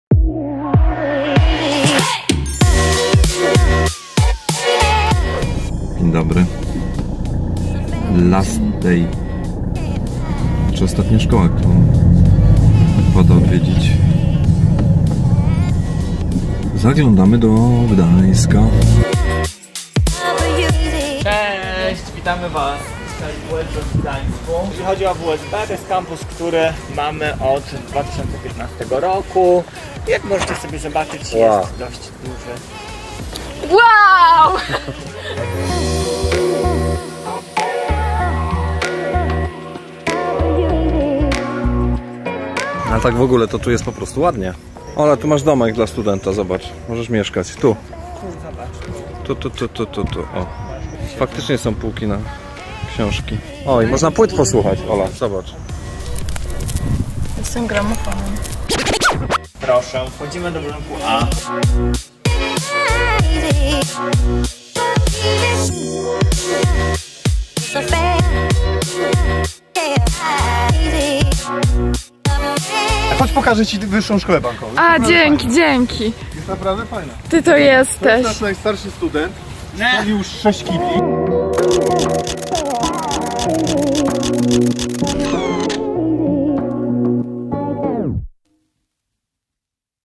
Wizyta w Gdańskim WSB i Czas Decyzji! – Gdańsk odc.9
Ostatni dzień akcji wjazdnaWSB to wizyta w gdańskim oddziale szkoły. Sprawdzamy, jak WSB wygląda od środka, zwiedzając dokładnie każde piętro budynku. A czym byłaby wizyta w Gdańsku bez popływania łodzią? Wychodząc z tego samego założenia, Ola i Marek wybrali się w rejs po Bałtyku. To właśnie tam Ola miała wreszcie chwilę, by w spokoju zastanowić się nad tym, jaką uczelnię wybrać… Zobacz, co nowego w Gdańsku!